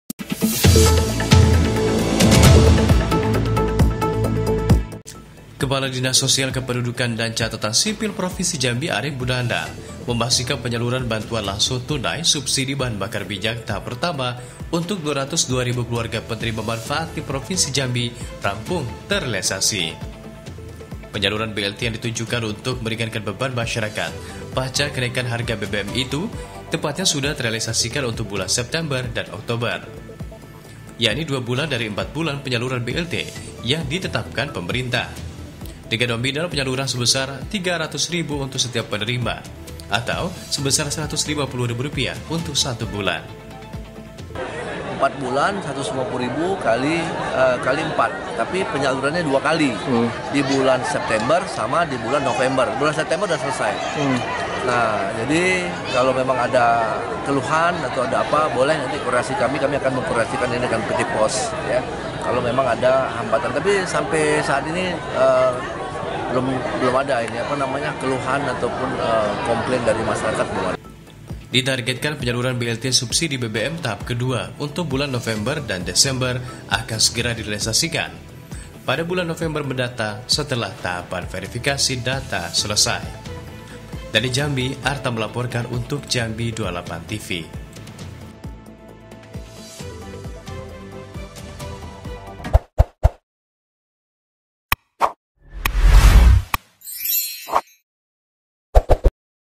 Penyaluran BLT Subsidi BBM Tahap Pertama di Jambi Rampung
JAMBI28TV, JAMBI - Dinas Sosial Kependudukan dan Pencatatan Sipil (Dinsosdukcapil) Provinsi Jambi berdasarkan laporan dari PT Pos Indonesia selaku pihak yang menyalurkan Bantuan Tunai Langsung Bahan Bakar Minyak (BLT BBM) ke masyarakat tahap pertama rampung terealisasi. Arta